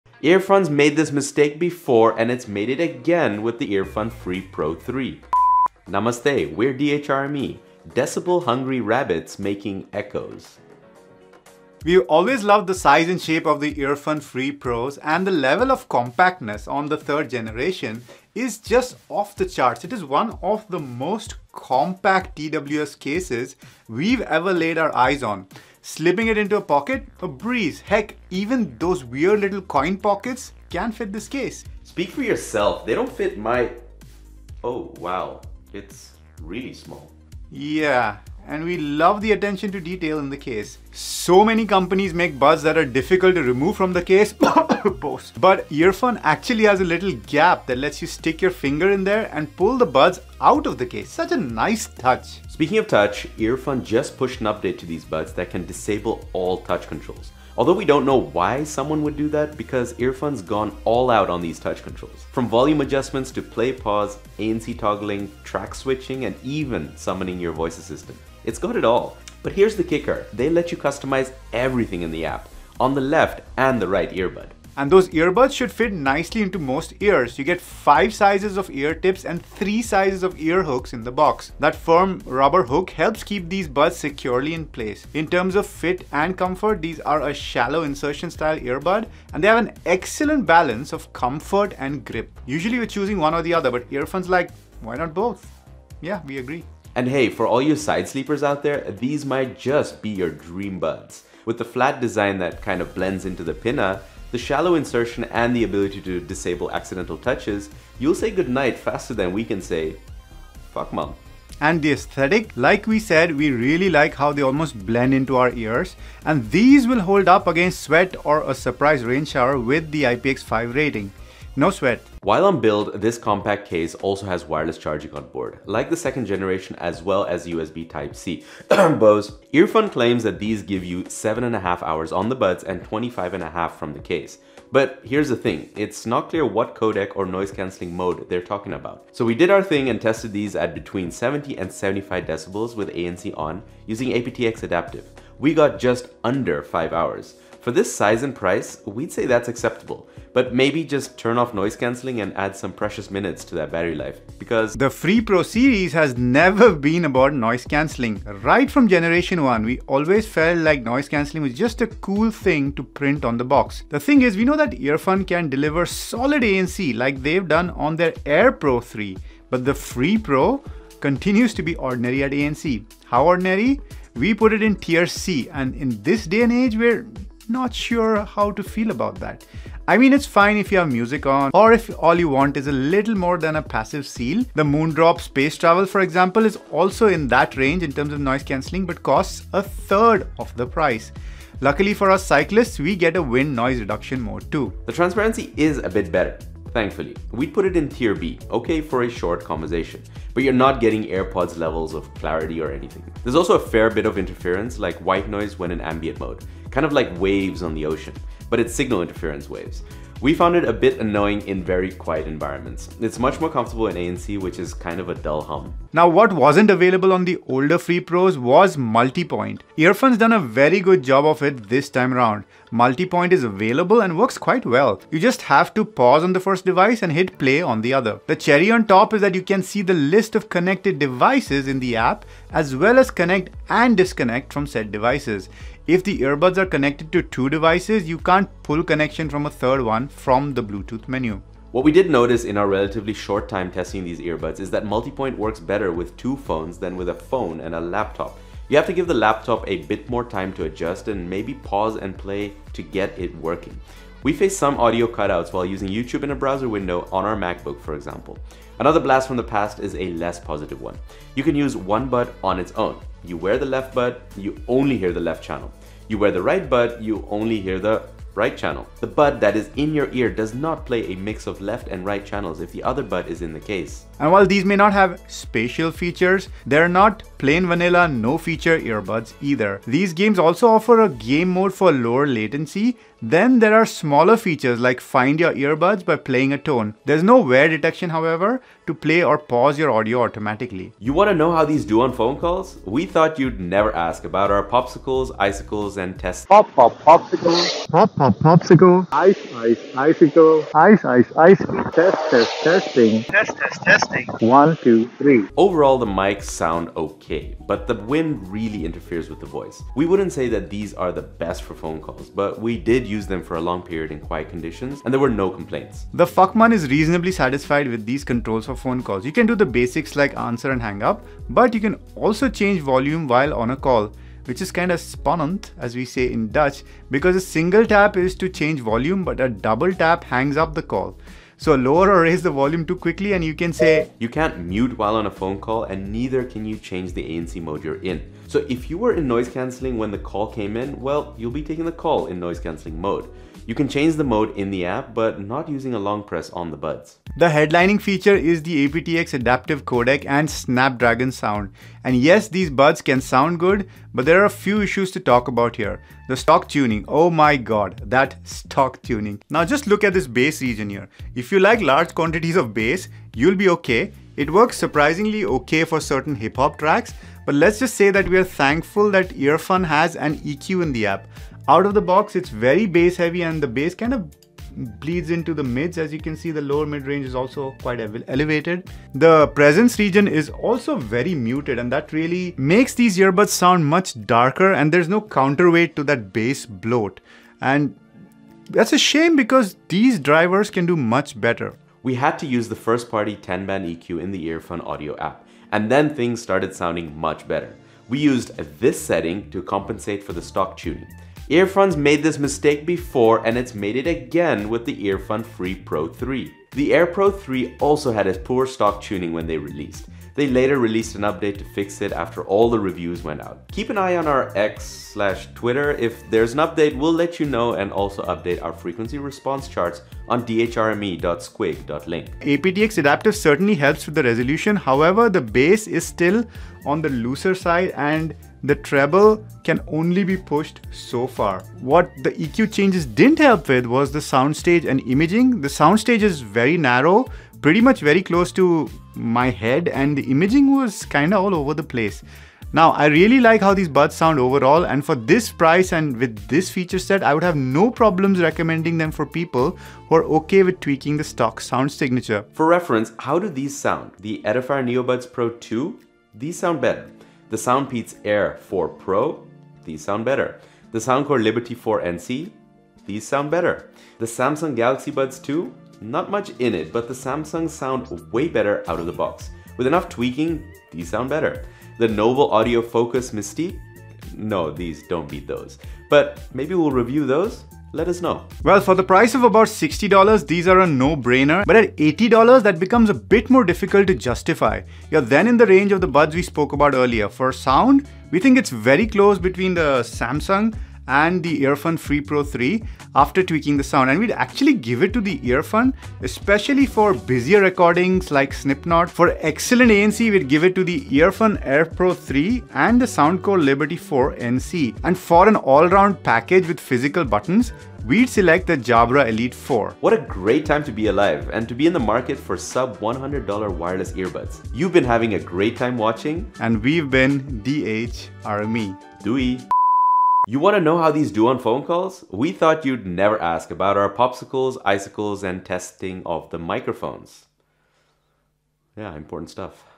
We Love it (Except For One Big Mistake) - EarFun Free Pro 3
Earfun’s made this mistake before… and it’s made it again with the EarFun Free Pro 3.

00:00 Introduction
00:11 Build Quality
02:02 Battery Life & Charging
02:35 ANC & Transparency
03:36 Extras
04:50 Microphones
05:47 Sound Quality
07:58 To buy or not to buy